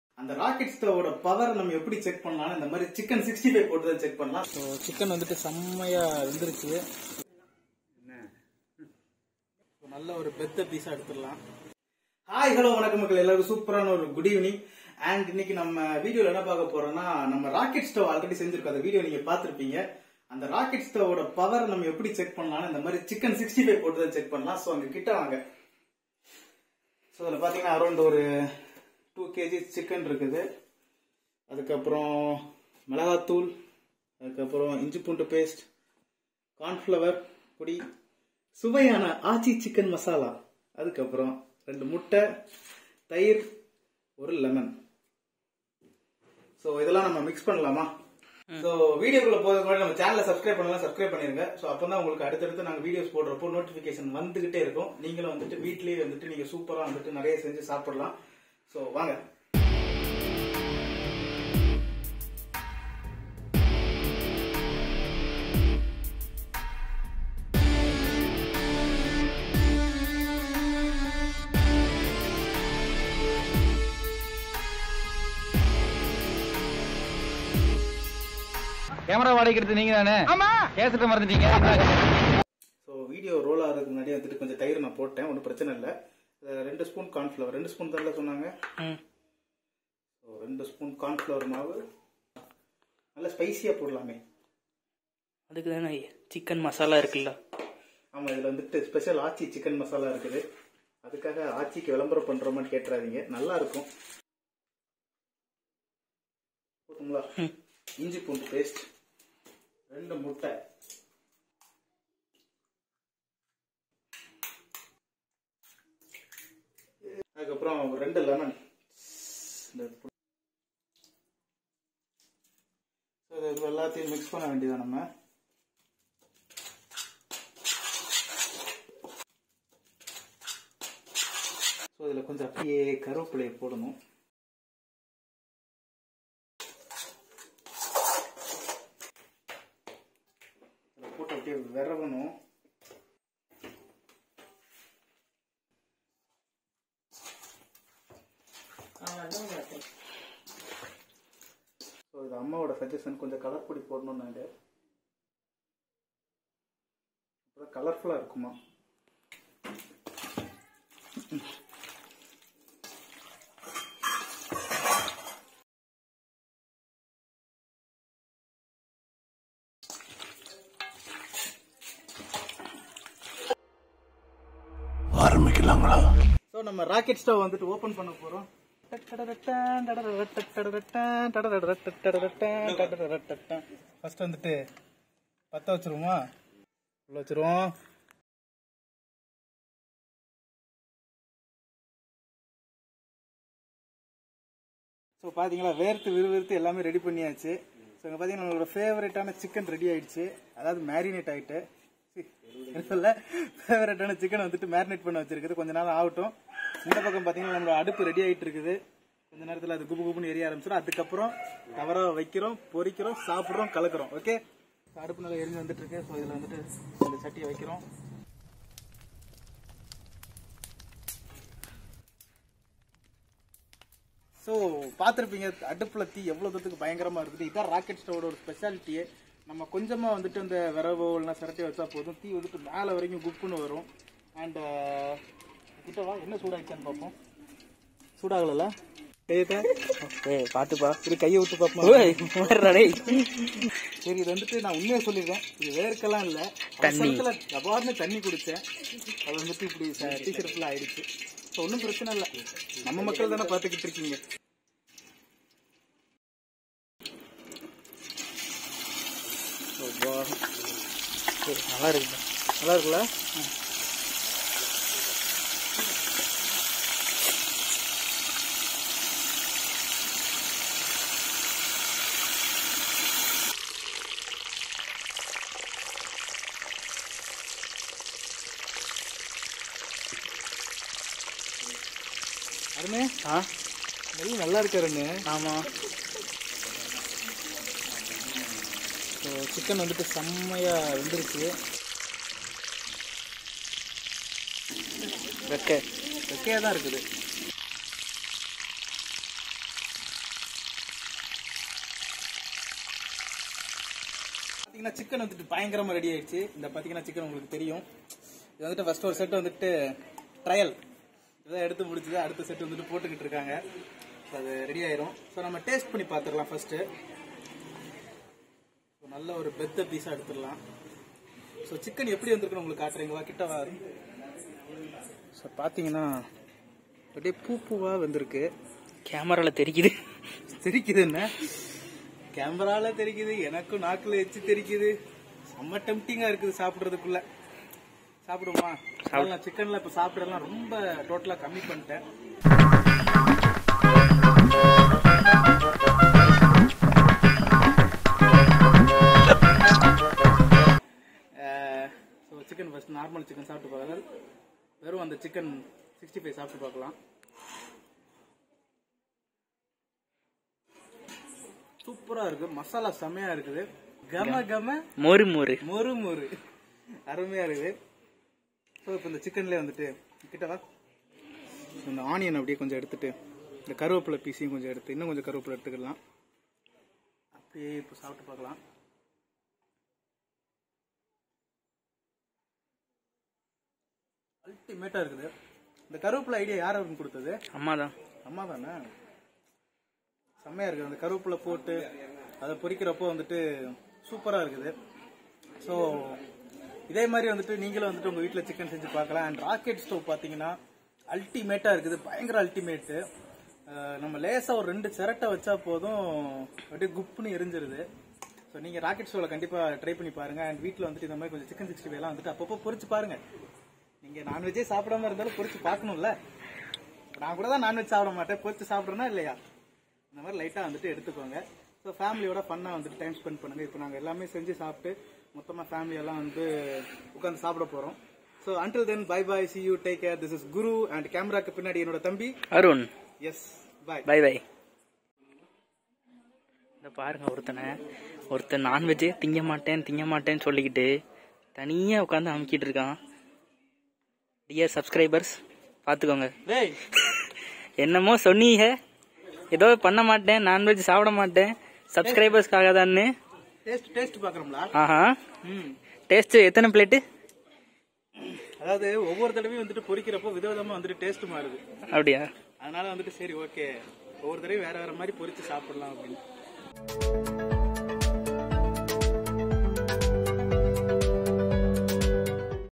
Chicken 65 செய்வது எப்படி? | Cooking in Rocket Stove | DIY Stove | Tamil
Chicken 65 in Rocket Stove!
Easy method to cook in Tamil..

Music Courtesy: Raj Bharath Music School